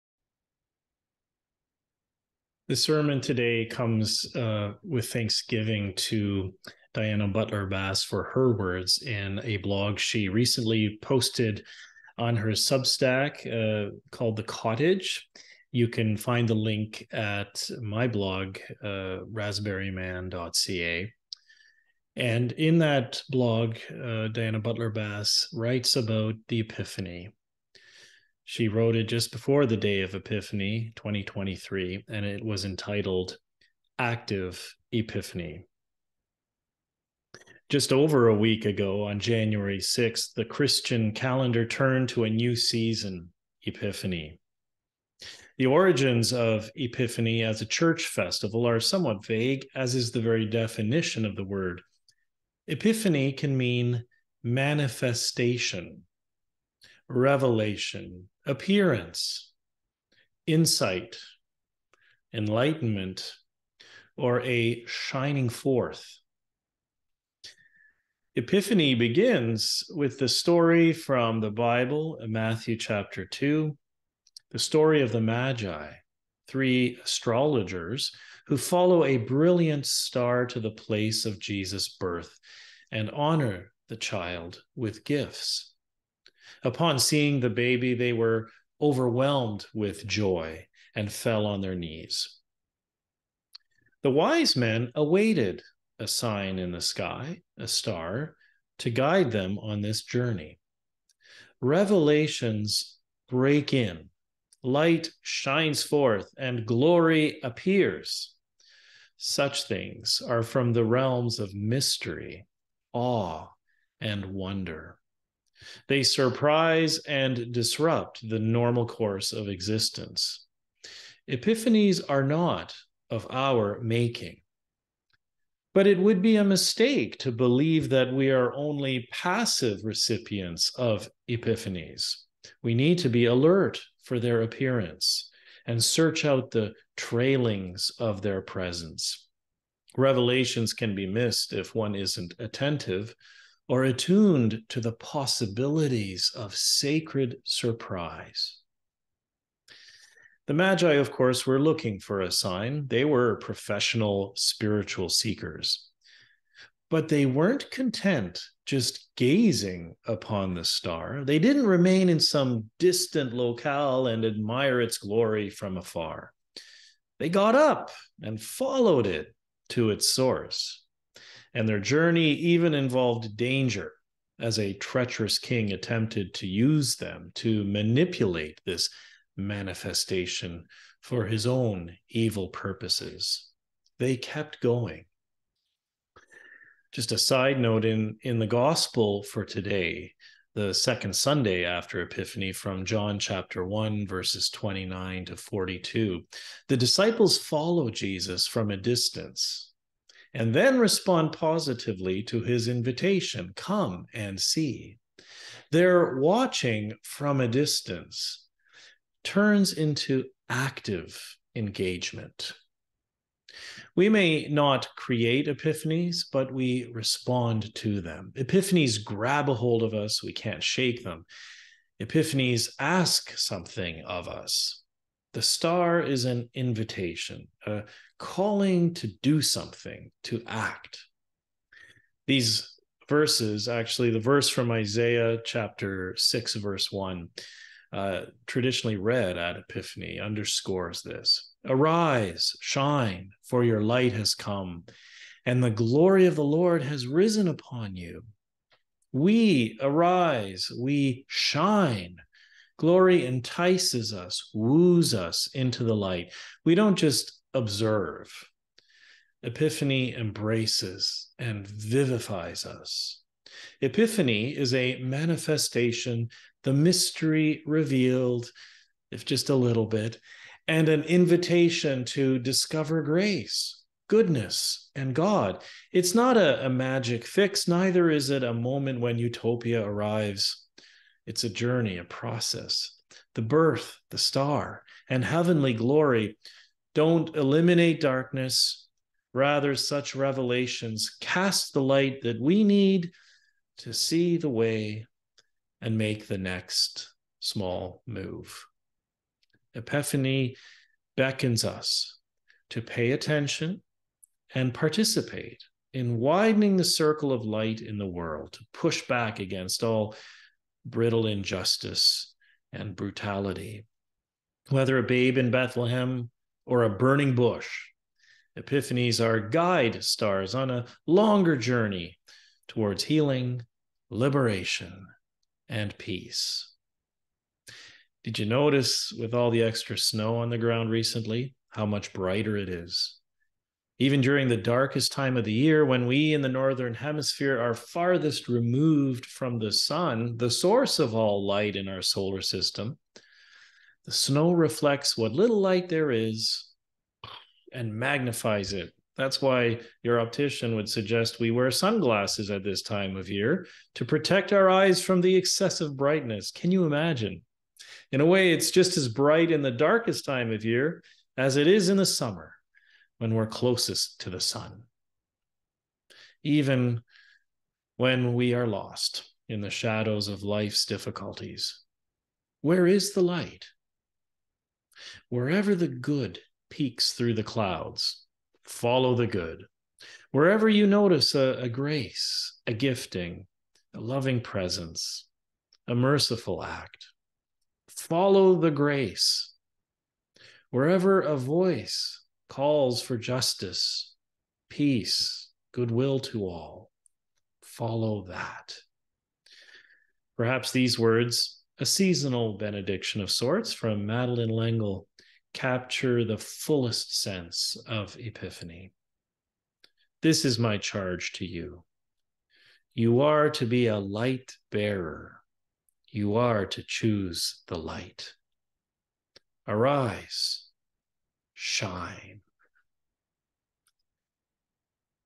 What will you do when God calls?
The season after Epiphany is a call from God to follow a star.  Wherever you perceive something good, wherever you see acts of justice, wherever someone practises mercy and grace -- these are the stars shining in the night sky. Thanks to Diana Butler Bass from her blog "The Cottage" where she writes about "Active Epiphany" (January 5, 2023)